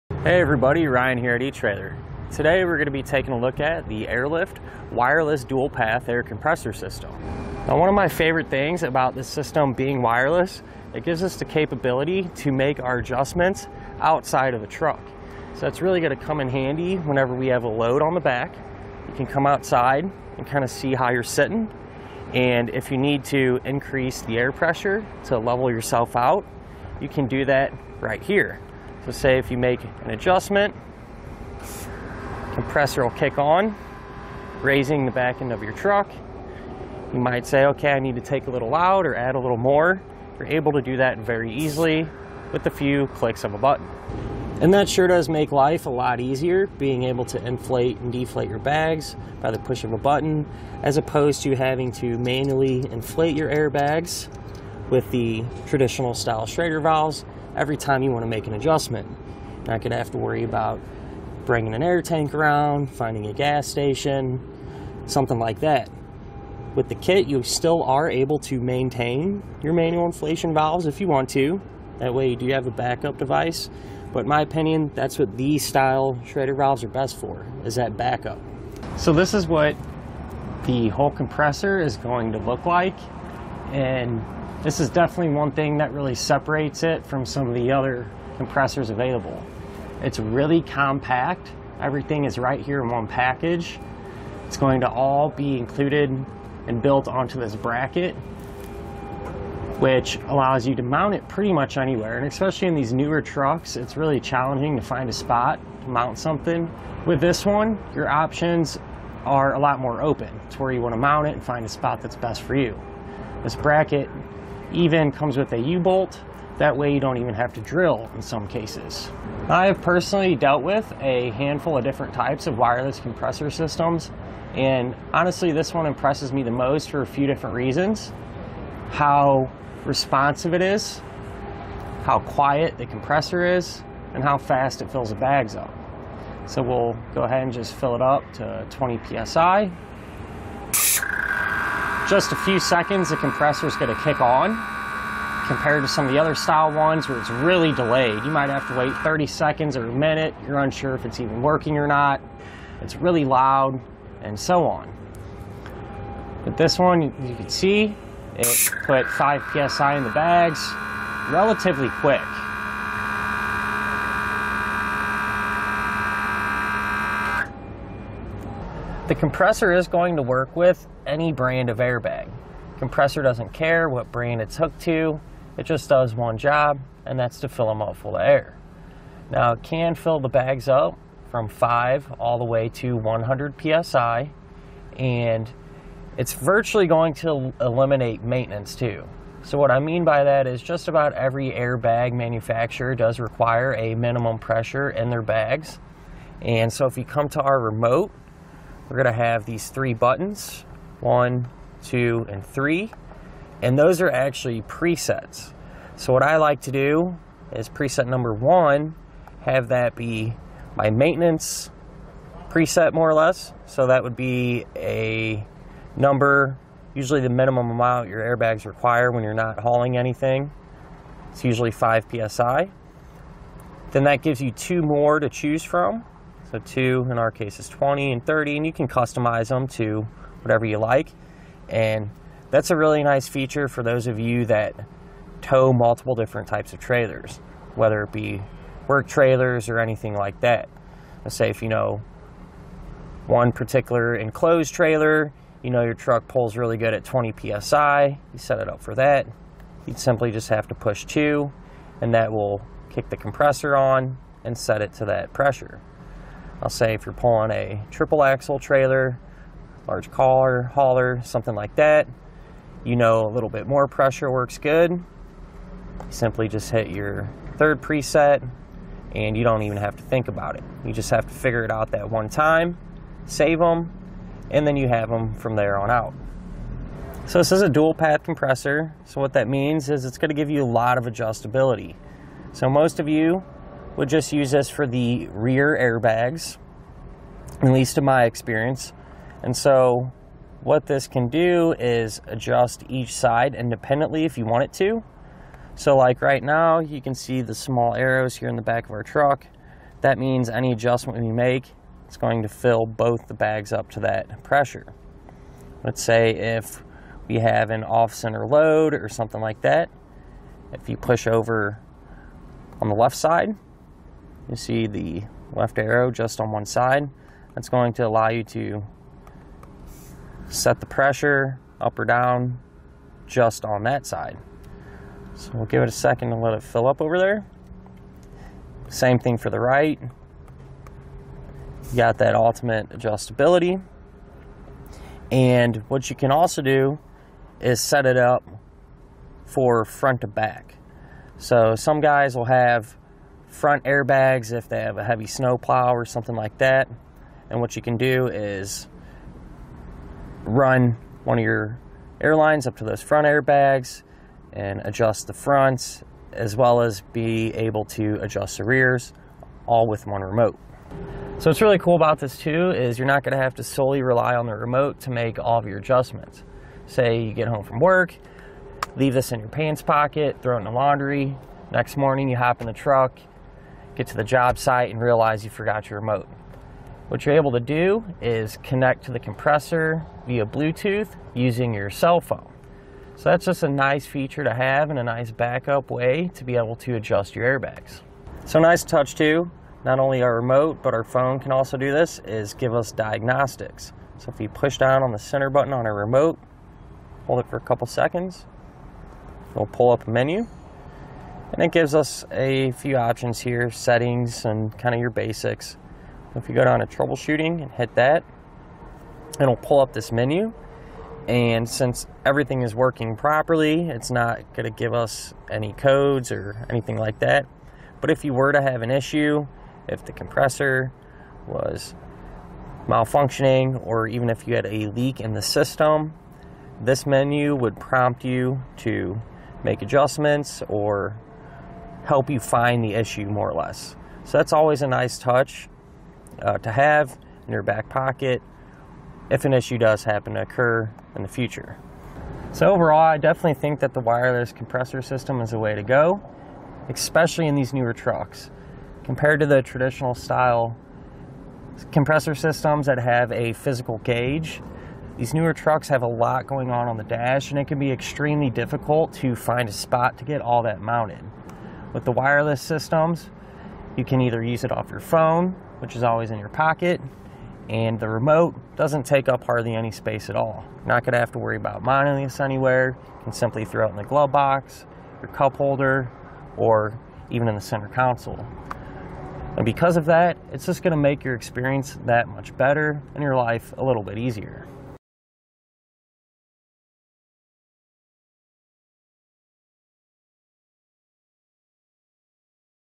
etrailer | Air Lift WirelessAIR Compressor System for Air Helper Springs Review
Click for more info and reviews of this Air Lift Air Suspension Compressor Kit:

Ryan:                                                  Hey everybody. Ryan here at etrailer. Today we're going to be taking a look at the Air Lift wireless dual path air compressor system. Now one of my favorite things about the system being wireless, it gives us the capability to make our adjustments outside of the truck. So that's really going to come in handy whenever we have a load on the back. You can come outside and kind of see how you're sitting and if you need to increase the air pressure to level yourself out, you can do that right here.

So say if you make an adjustment, compressor will kick on raising the back end of your truck. You might say, "Okay. I need to take a little out or add a little more," you're able to do that very easily with a few clicks of a button. And that sure does make life a lot easier, being able to inflate and deflate your bags by the push of a button, as opposed to having to manually inflate your airbags with the traditional style Schrader valves every time you want to make an adjustment.Not going to have to worry about bringing an air tank around, finding a gas station, something like that. With the kit, you still are able to maintain your manual inflation valves if you want to that way you do have a backup device.

But in my opinion, that's what these style Schrader valves are best for is that backup. So this is what the whole compressor is going to look like. And this is definitely one thing that really separates it from some of the other compressors available. It's really compact. Everything is right here in one package.

It's going to all be included and built onto this bracket, which allows you to mount it pretty much anywhere.And especially in these newer trucks, it's really challenging to find a spot to mount something. With this one your options are a lot more open. It's where you want to mount it and find a spot that's best for you. This bracket even comes with a U bolt that way you don't even have to drill in some cases. I have personally dealt with a handful of different types of wireless compressor systems and honestly this one impresses me the most for a few different reasons.

How responsive it is, how quiet the compressor is, and how fast it fills the bags up. So we'll go ahead and just fill it up to 20 PSI.Just a few seconds the compressor's going to take on compared to some of the other style ones where it's really delayed. You might have to wait 30 seconds or a minute, you're unsure if it's even working or not, it's really loud, and so on. With this one, you can see it put five PSI in the bags relatively quick. The compressor is going to work with any brand of airbag. Compressor doesn't care what brand it's hooked to. It just does one job and that's to fill them all full of air. Now it can fill the bags up from five all the way to 100 PSI and it's virtually going to eliminate maintenance too. So what I mean by that is just about every airbag manufacturer does require a minimum pressure in their bags. And so if you come to our remote, we're going to have these three buttons, one, two, and three, and those are actually presets.So what I like to do is preset number one have that be my maintenance preset, more or less. So that would be a number, usually the minimum amount your airbags require when you're not hauling anything. It's usually five PSI. Then that gives you two more to choose from. So two in our case is 20 and 30 and you can customize them to whatever you like. And that's a really nice feature for those of you that tow multiple different types of trailers, whether it be work trailers or anything like that. Let's say if you know one particular enclosed trailer, you know your truck pulls really good at 20 PSI, you set it up for that. You'd simply just have to push two and that will kick the compressor on and set it to that pressure. I'll say, if you're pulling a triple axle trailer, large car hauler, something like that, you know a little bit more pressure works good.Simply just hit your third preset and you don't even have to think about it. You just have to figure it out that one time, save them, and then you have them from there on out. So this is a dual path compressor. So what that means is it's going to give you a lot of adjustability. So most of you would just use this for the rear airbags, at least in my experience. And so what this can do is adjust each side independently, if you want it to. So like right